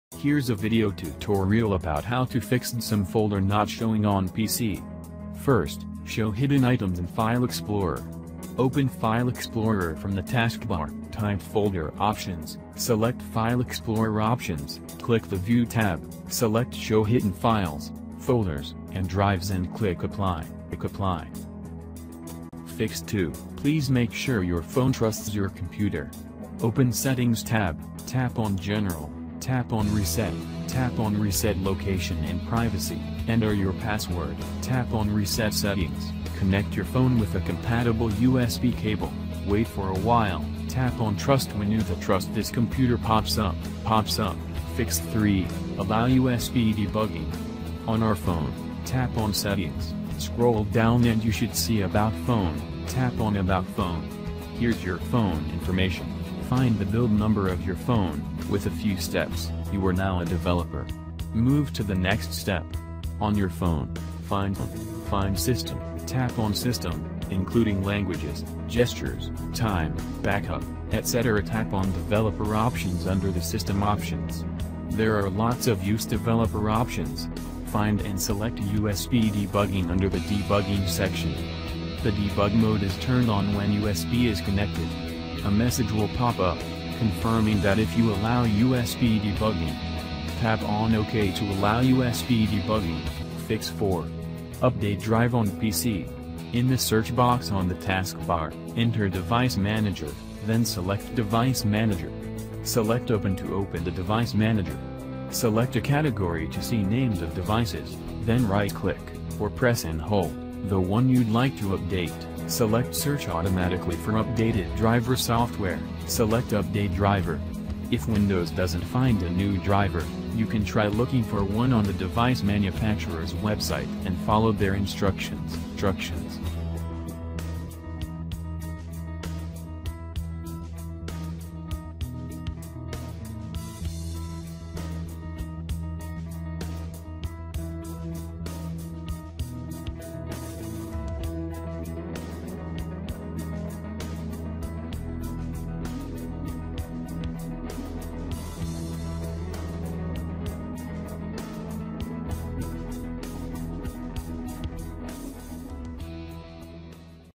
How to Easily Fix DCIM Folder Not Showing on PC
What to do to fix DCIM folder not showing on PC issue?
Here are four easy fixes to DCIM folder not showing on PC:

1. Show hidden items in File Explorer.
2. Make sure your phone trusts your computer.
3. Allow USB debugging on your phone.
4. Update drive on PC.